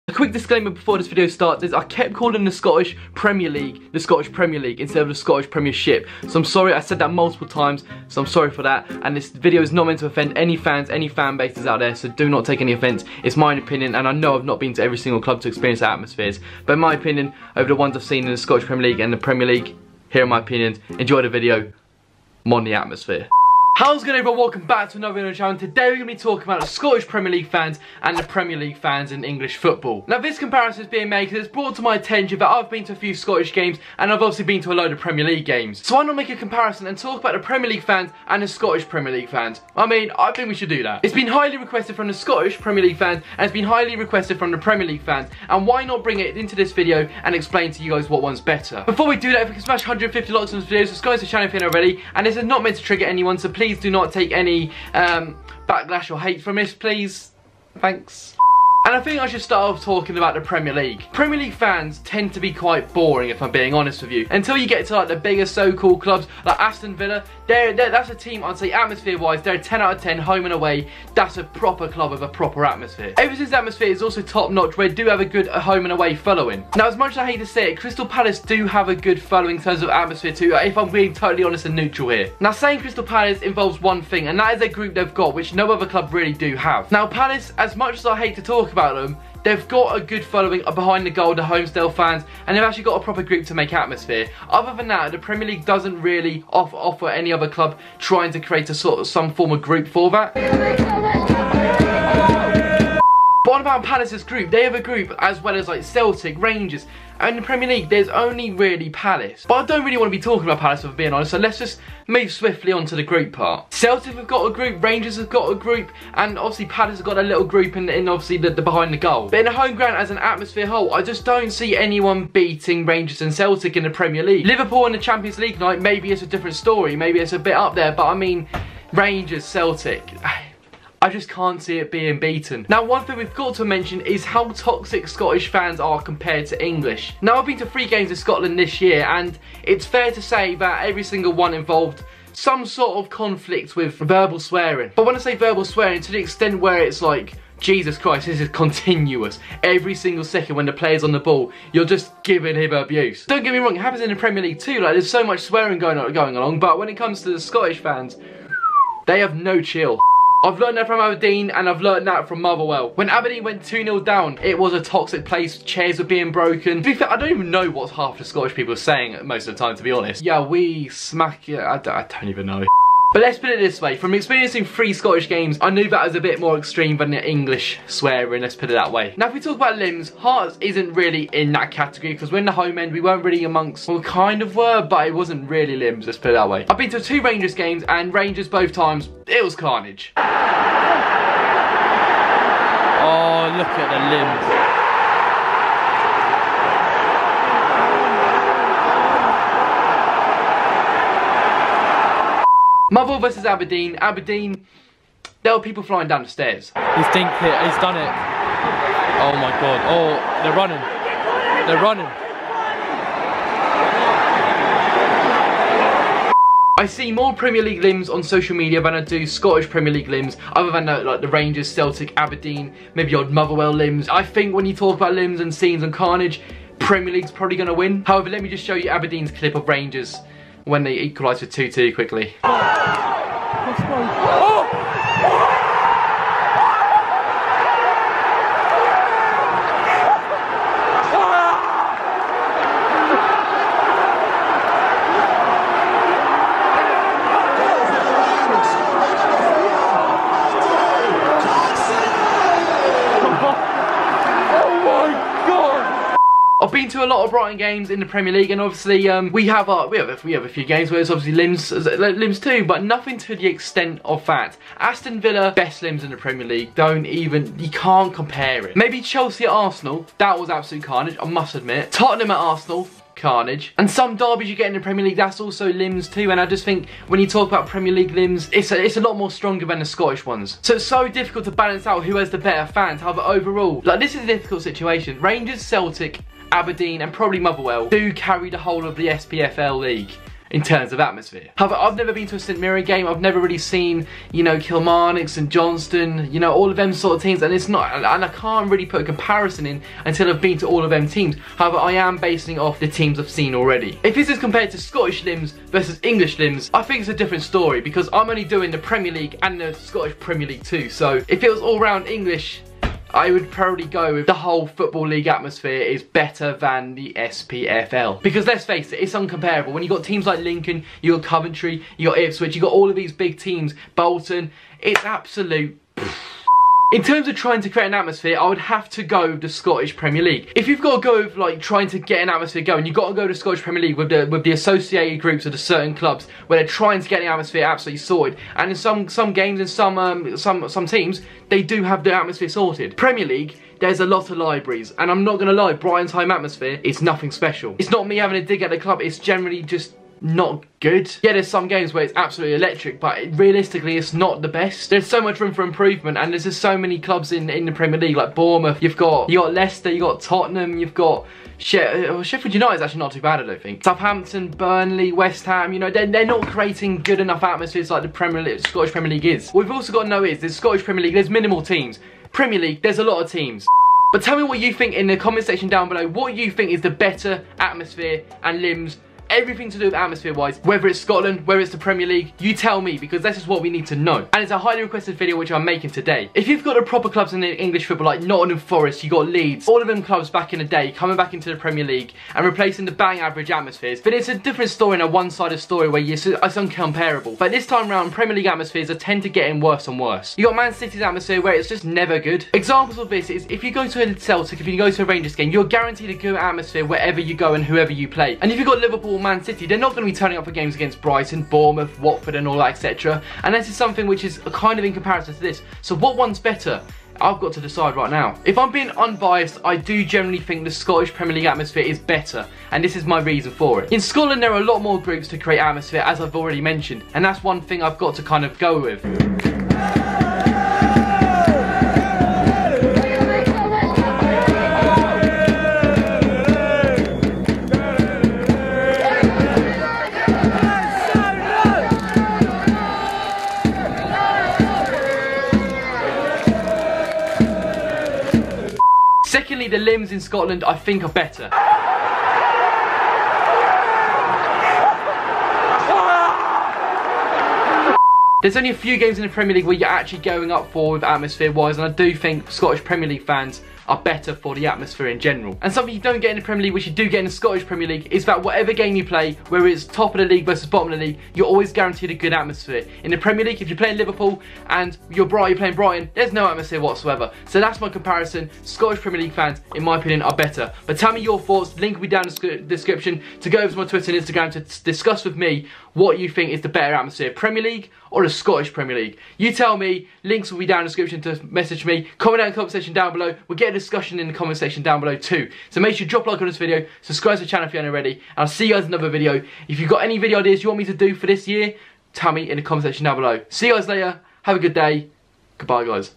RANGERS or LIVERPOOL?? - Scottish Vs English Premiership Atmospheres?!
Hope you guys enjoyed the video! Today i discussed the comparrisions between the scottish premiership atmosphere and the premier league atmosphere. Rangers or liverpool? we decide in this video!